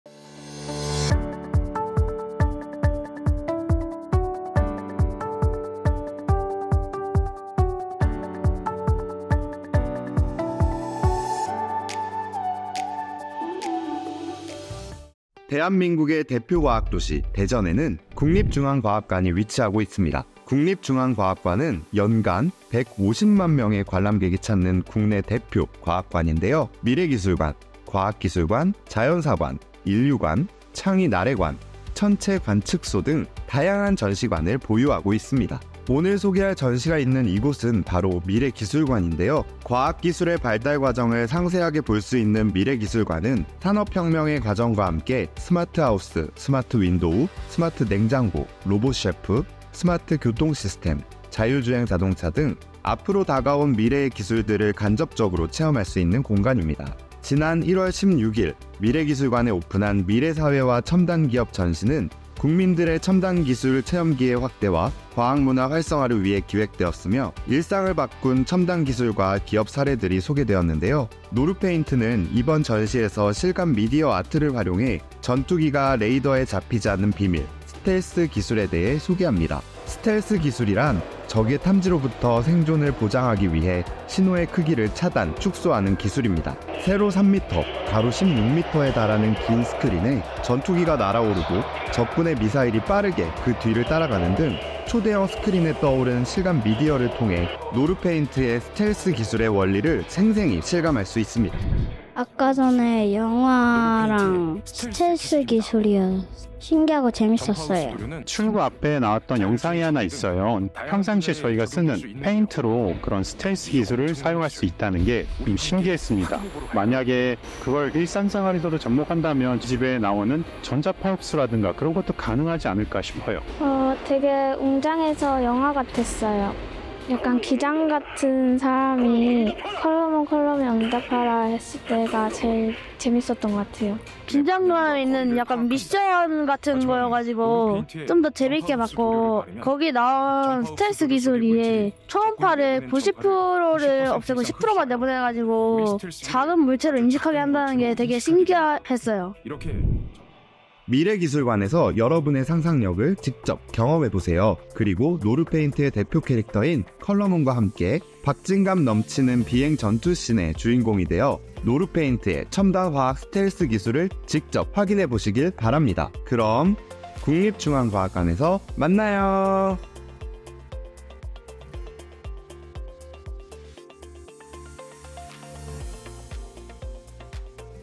[SUB] 대전 국립중앙과학관에 스케일 미친 무료 미디어 아트 전시 등장! 노루페인트의 스텔스 기술 소개 (Stealth Technique)
"컬러몬 응답하라! 컬러몬 응답하라!"
대전에 위치한 국립중앙과학관에 노루가 나타났습니다!
노루는 이번 전시에서 적의 탐지로부터 생존을 보장하는 '스텔스 기술'에 대해 소개하는데요.
짧은 유튜브 영상으로는 감히 다 담을 수 없는 이번 전시,
24년 12월 말까지 무료로 볼 수 있다고 하니 당장 가봐야겠죠?

* 전시 정보
일정 : 24.1.16 ~ 24.12.29
위치 : 국립중앙과학관 내 미래기술관 2층